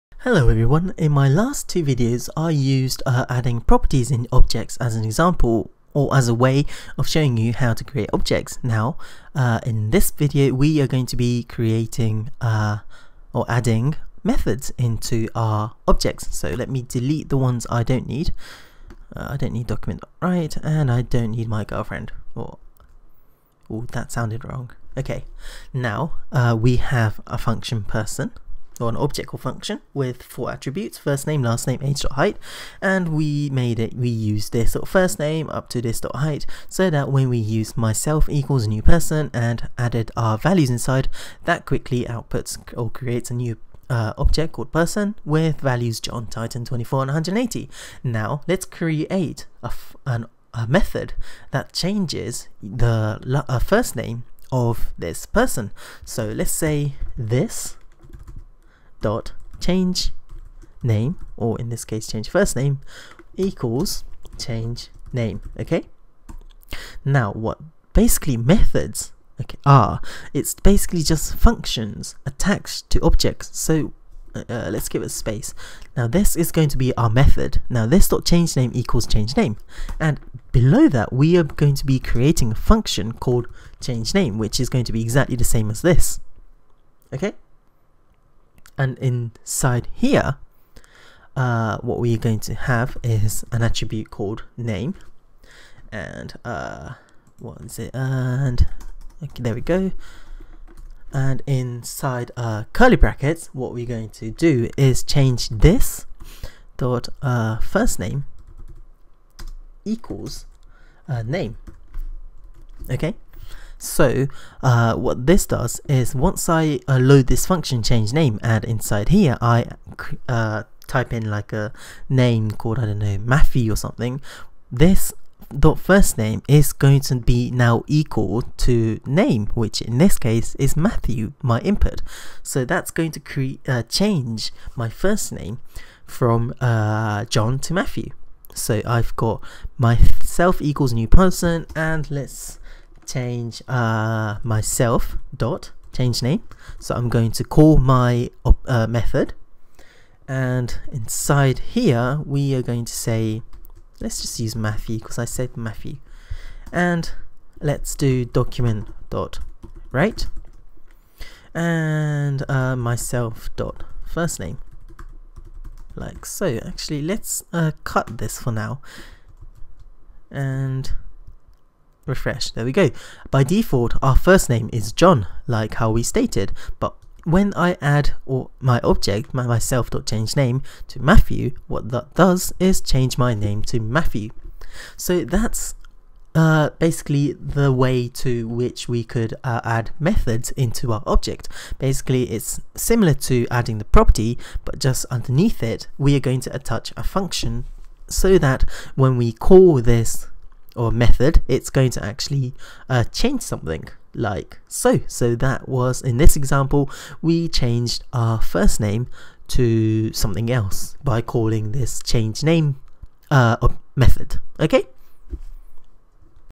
JavaScript - 35. Adding methods to JavaScript objects
In this video, we add methods to our JavaScript objects. Adding methods is similar to adding properties, except we need to stick a function underneath it in order for the method to behave how we want to.